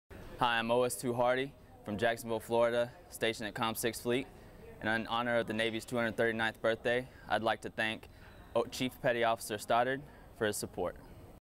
Navy's 239th Birthday Celebration - Thanking those who support us
This celebration is an opportunity to show the importance of the Navy’s history and heritage while we recognize past and present accomplishments of Sailors in warfighting efforts. Warfighting First is the first of the Navy’s tenets and is the key consideration applied to every decision made in the Navy. The Navy will continue to be at the front line of our nation’s warfighting efforts in war and peace. We are proud of our heritage of success in battle on, above, and below the sea. It couldn’t be done without support from the homefront.”